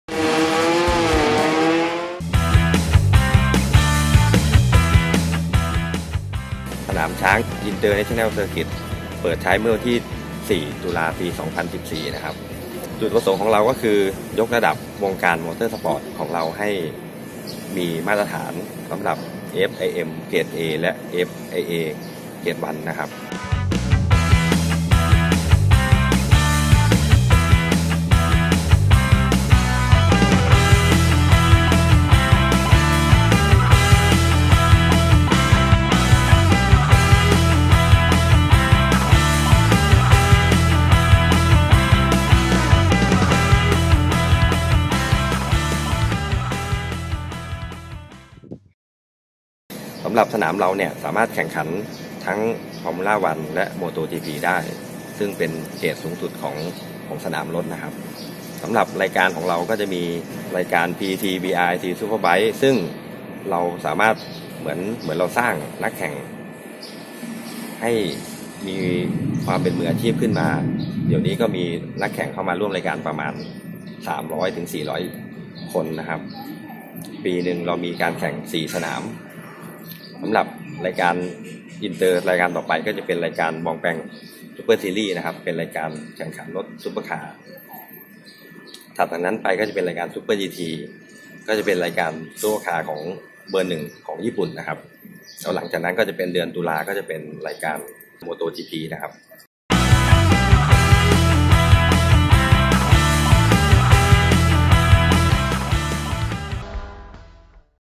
Chang International Circuit
การท่องเที่ยวแห่งประเทศไทย สำนักานจังหวัดสุรินทร์  นำสื่อมวลชนเยี่ยมชมสนาม Chang International Circuit  จ.บุรีรัมย์  มีห้องรับกล้องวงจรปิด  เพื่อควบคุมดูแลความเรียบร้อยในการแข่งขันที่สร้างด้วย 500 ล้านบาท  เป็นสนามมาตรฐานที่ใช้แข่งขันรถจักรยนต์ระดับโลกได้  อัฒจันทร์  สามารถมองเห็นการการแข่งขันได้ทุกโค้ง